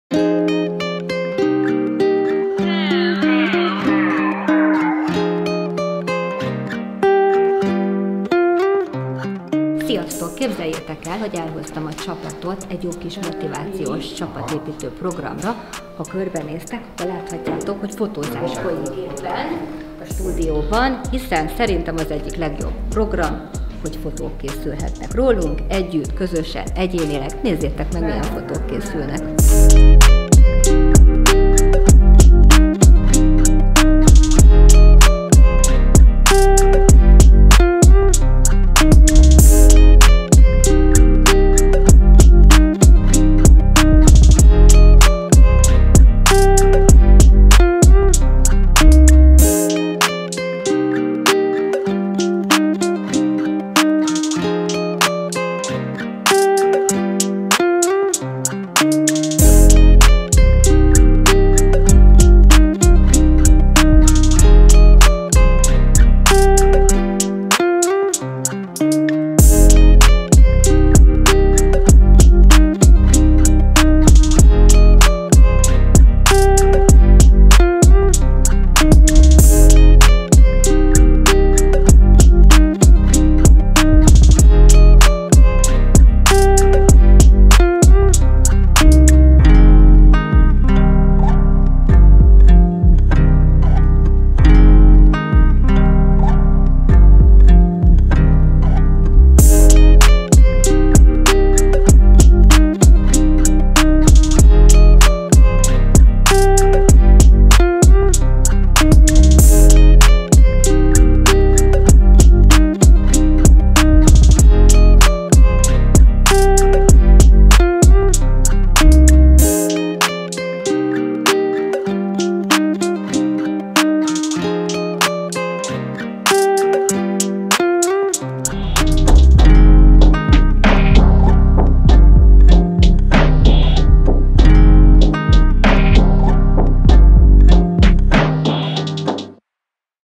EverDerm csapatépítő - fotózás
Nézzetek be az EverDerm csapatépítőbe!
Egy fotózásra mentünk el, volt minden, smink, haj, csodás ruhák és ennek eredményeképpen meglettek a tökéletes képek is!